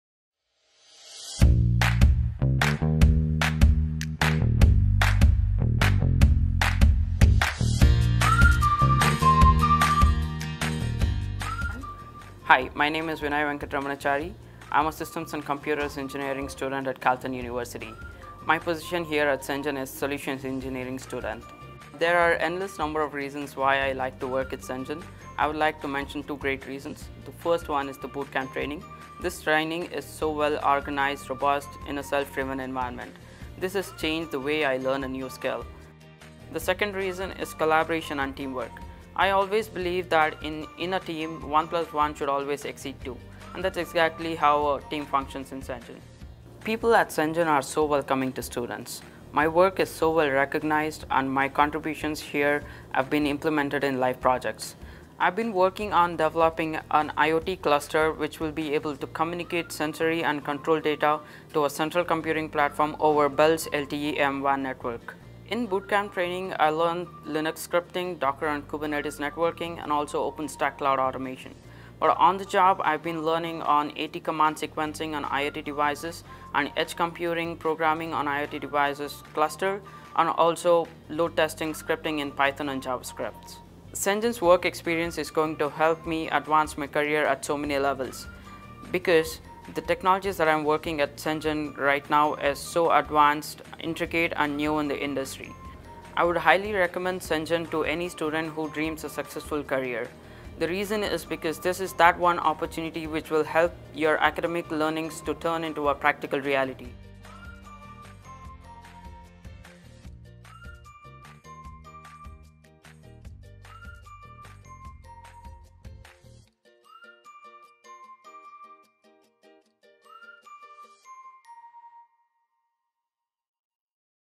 Student Spotlight: Vinay Venkataramana Chary
Vinay is an Electrical and Computer engineering student at Carleton University who worked as a Solutions Engineering Student.

In this iteration of our Student Spotlight series, Vinay tells us about his favourite moments of working at CENGN, how he has contributed to the CENGN ecosystem, and what he's gained from his experience as a co-op student at CENGN.